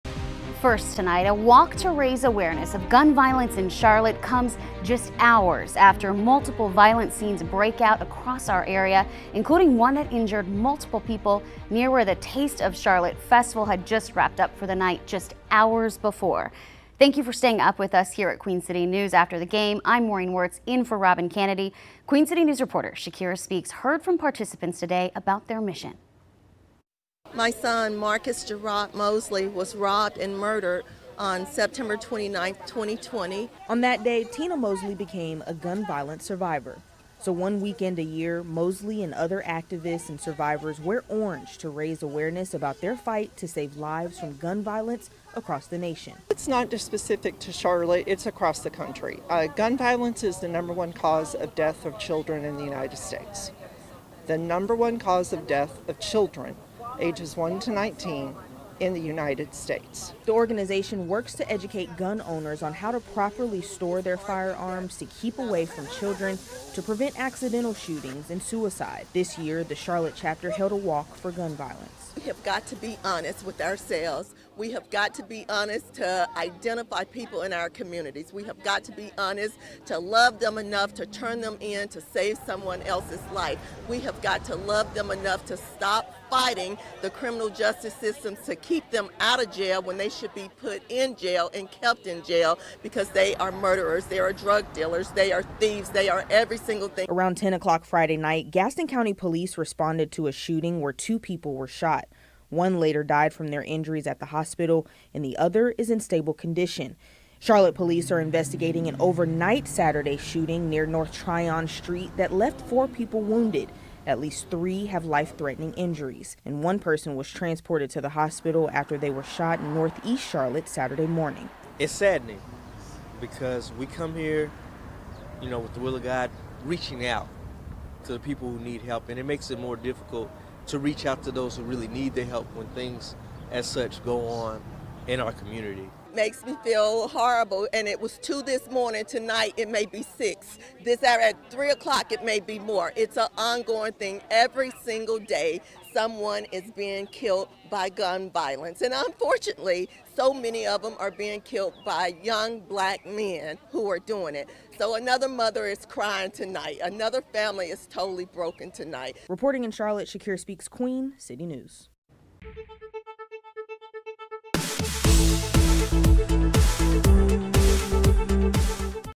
Walk raises awareness of gun violence in Charlotte
Just hours after several shootings in Charlotte, a walk was held to bring awareness to gun violence in Charlotte.